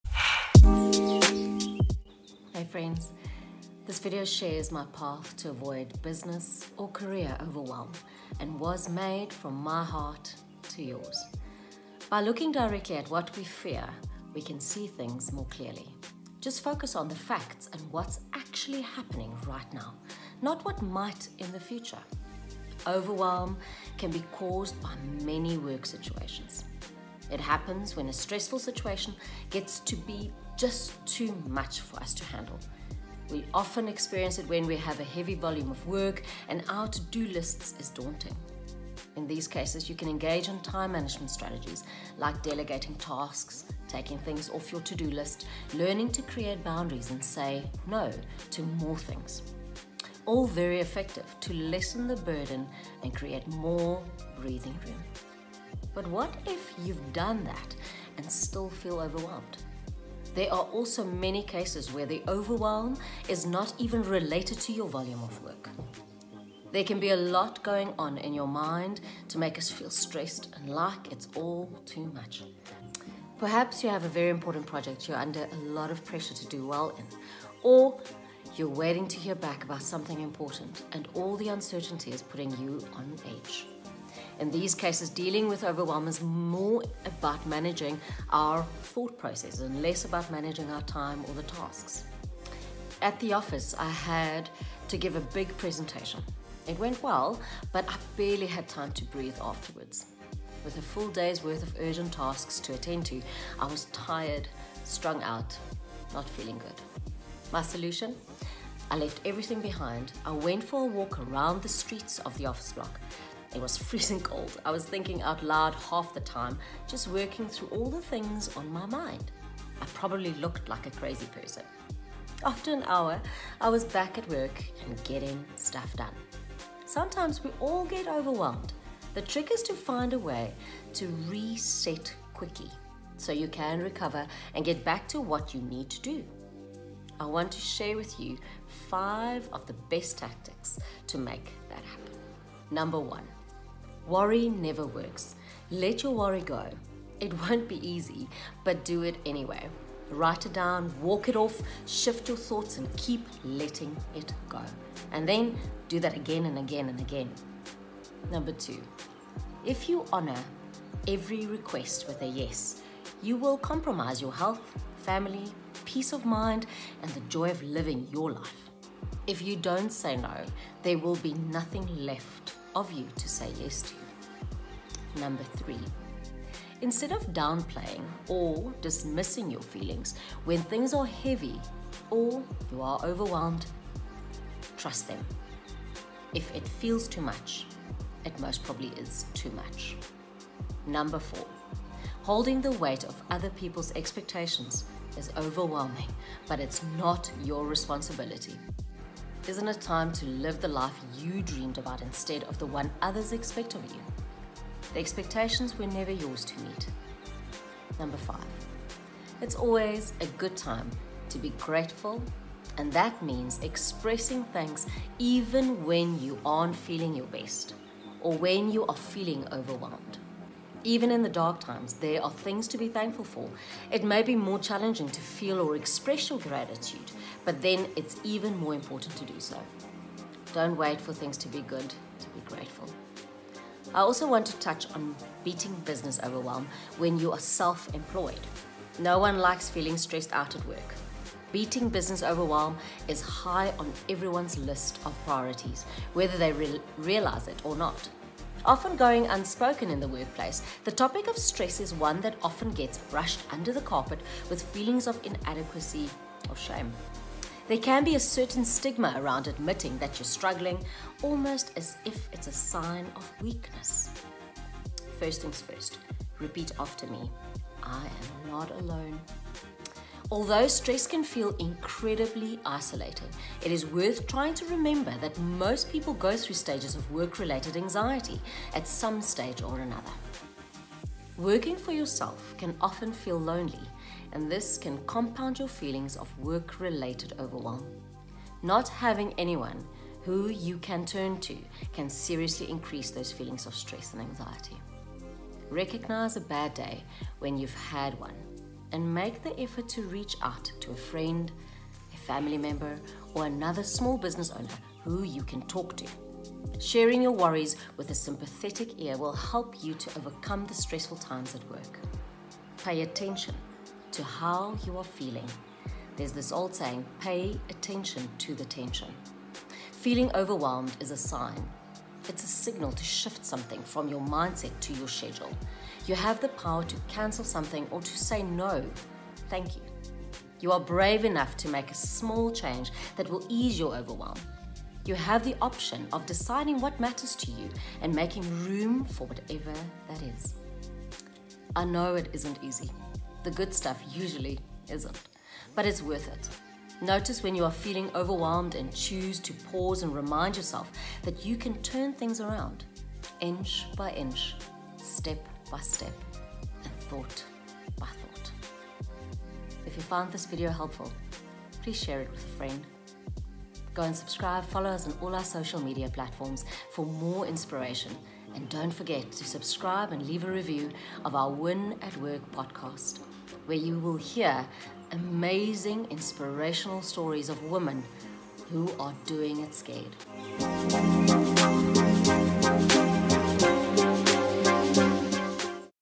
How to Avoid Business & Career Overwhelm
This video shares my path to avoid business or career overwhelm...and was made from my heart to yours.

By looking directly at what we fear we can see things more clearly.  Just focus on the facts and what’s actually happening right now, not what might in the future.

Overwhelm can be caused by many work situations. It happens when a stressful situation gets to be just too much for us to handle. We often experience it when we have a heavy volume of work and our to-do list is daunting. In these cases you can engage in time management strategies like delegating tasks, taking things off your to-do list, learning to create boundaries and say ‘No’ to more things - all very effective to lessen the burden and create more breathing room.